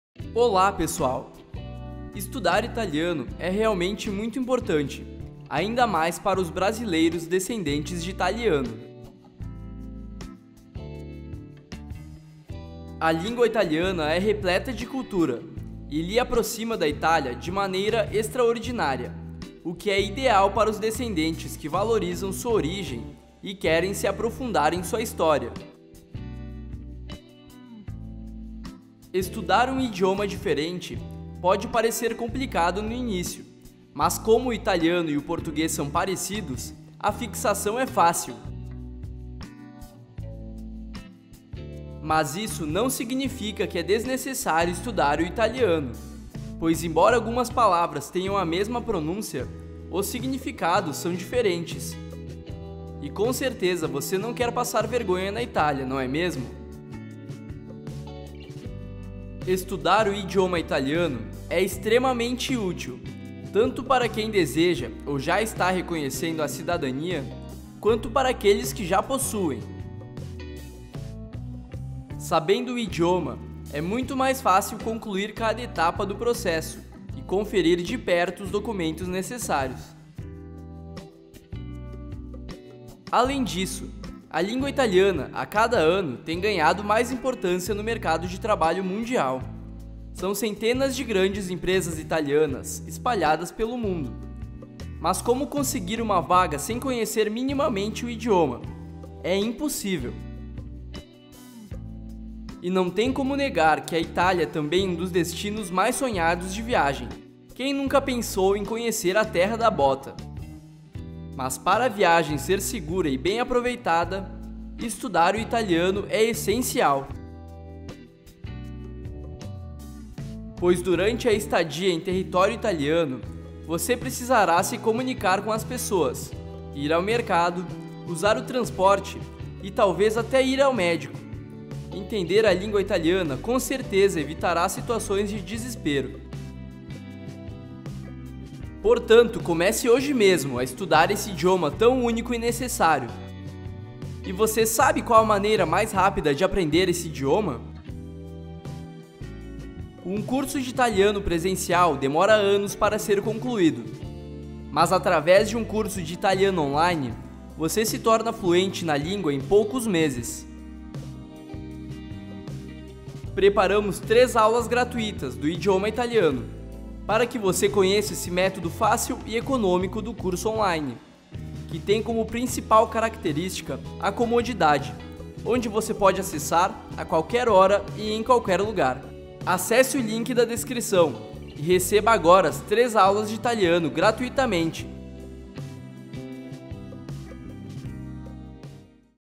ONDE ESTUDAR ITALIANO PARA CIDADANIA ITALIANA
Onde estudar italiano para cidadania italiana na Itália ou italiano para naturalização italiana por casamento ou viagem?

Acesse mais benefícios como empregos, bolsas-de-estudos, morar na Itália sabendo falar italiano! Veja mais detalhes aqui: